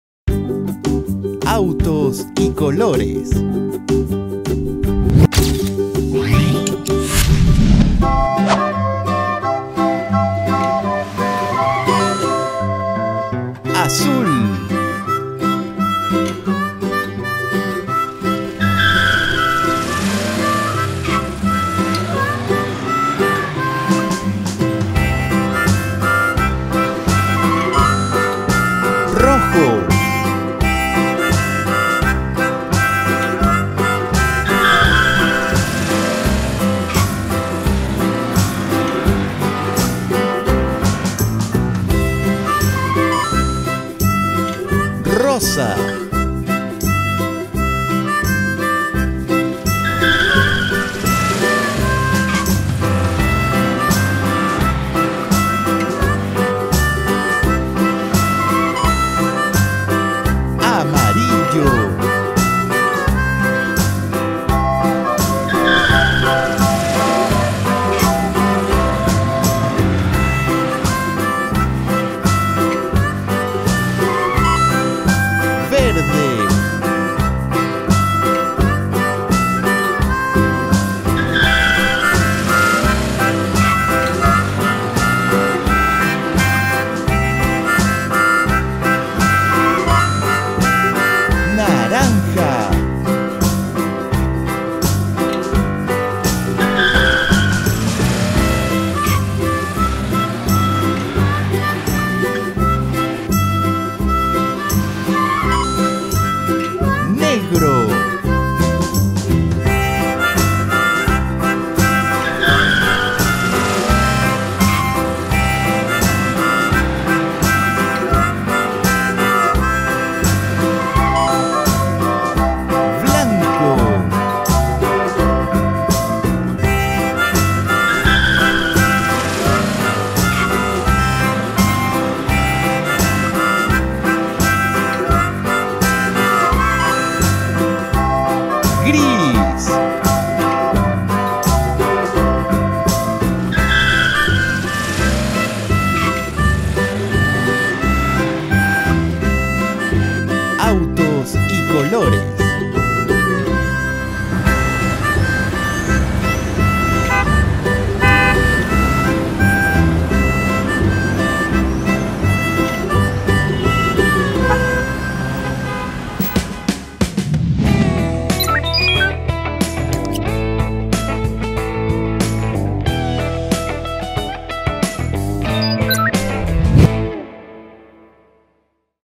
APRENDE los COLORES con AUTOS y COLORES 🚗
Aprende los colores de forma divertida, Autos y colores!, ¿de qué color es el auto?...Muy bien!, aprenderás los colores básicos, los peques se divierten y aprenden! les encanta verlo una y otra vez.

También estamos en instagram @nosvemosperejil

Aprende y diviértete!